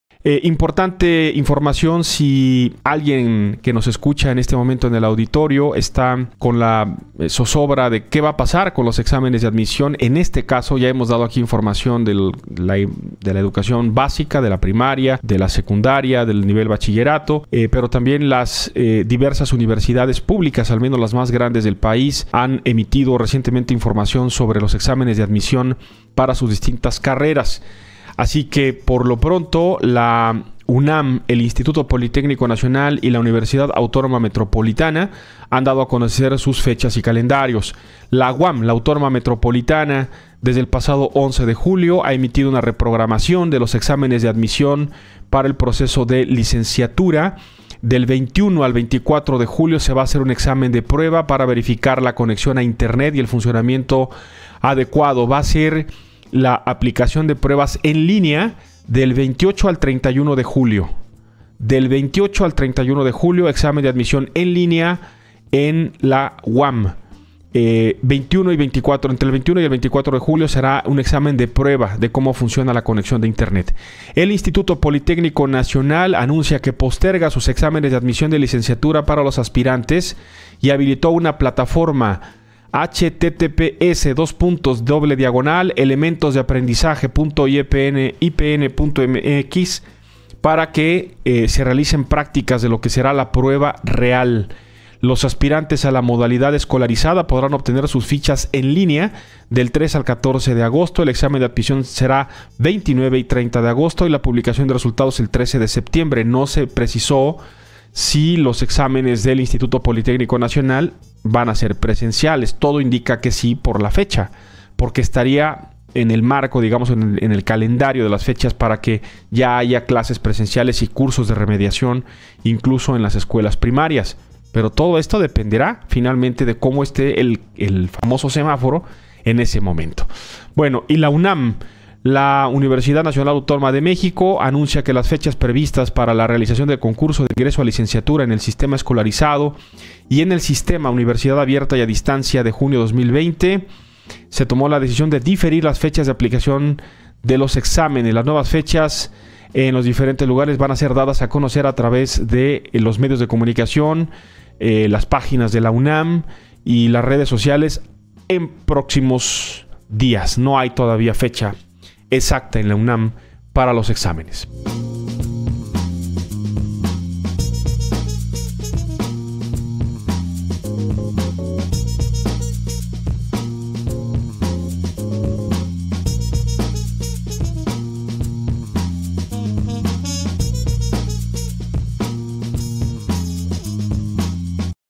Universidades ajustan fechas de exámenes de admisión
Debido a la pandemia, la forma en la que vivimos deberá ser replanteada, incluyendo a la educación tradicional en todos niveles, surgiendo por ahora la interrogante de cómo serán los exámenes de selección para acceder a la universidades públicas del país como la UNAM, IPN o UAM.